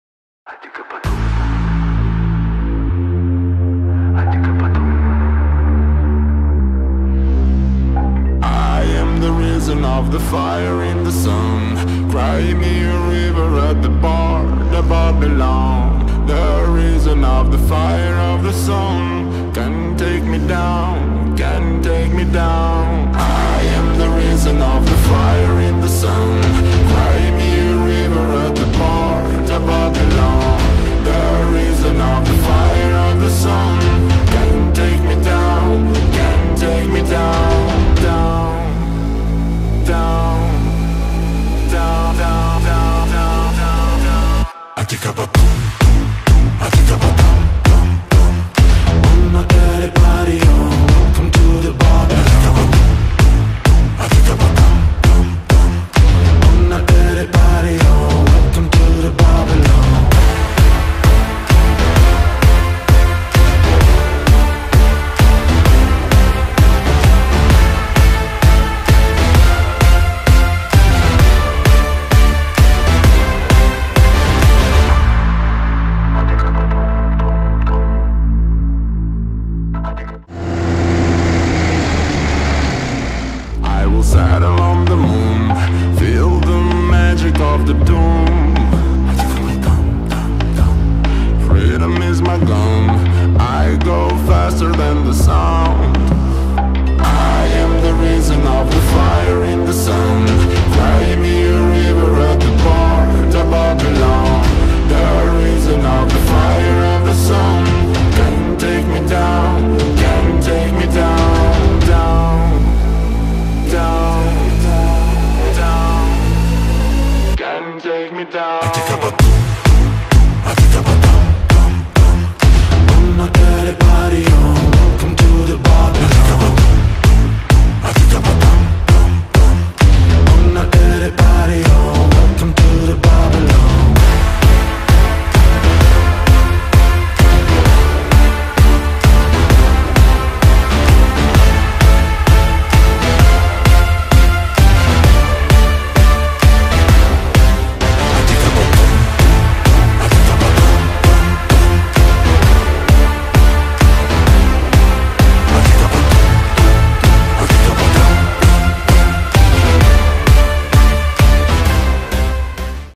🚜Siew 2020🚜 Wtopa?🚜 Ursus w Akcji?!🚜
Nagrywane: Gopro hero 7 silver

Maszyny zawarte w filmie:
U-R-S-U-S 1614 turbo
Grano system shark 3.0 + hydropack
Amazone D8- 30 Super

Muzyka nie jest moją własnością, nie przypisuję sobie do niej praw.